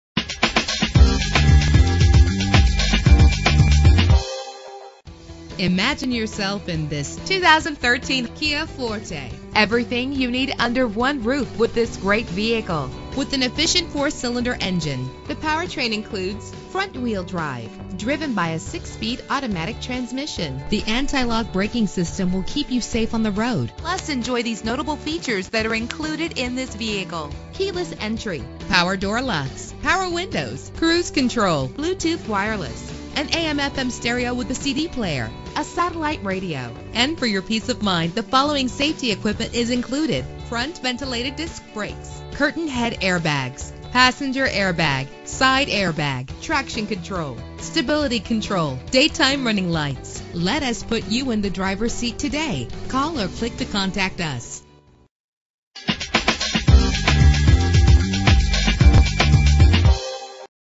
2013 Kia Forte - Ocala FL
Year: 2013
Make: Kia
Model: Forte
Trim: EX
Engine: 2.0 liter inline 4 cylinder 16 valve
Transmission: 6-Speed Automatic
Color: Bright Silver
Mileage: 37386
Address:
3440 South Pine Avenue